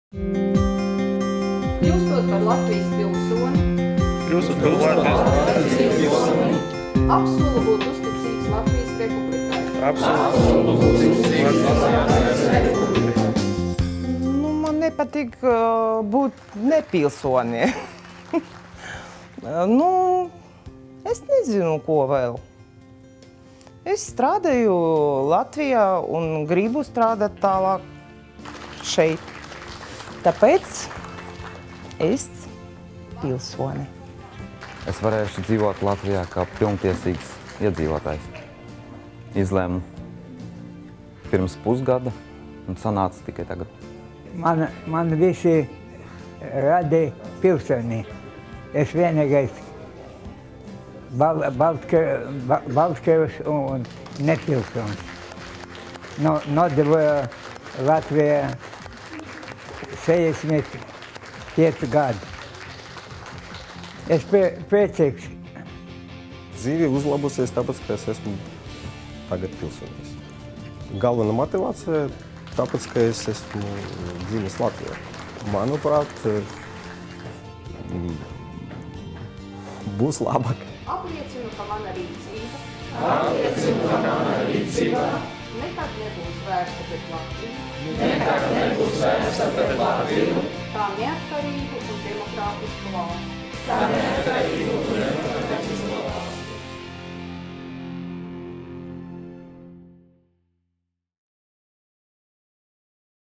Jauno Latvijas pilsoņu zvēresta nodošana
23.aprīlī notika svinīgā ceremonija, kurā solījumu par uzticību Latvijas Republikai deva 40 Latvijas pilsonības pretendenti, 6 dažādu tautību pārstāvji. Ar solījuma došanu un parakstīšanu noslēgsies Latvijas pilsonības pretendentu naturalizācijas iesnieguma izskatīšana Pilsonības un migrācijas lietu pārvaldē.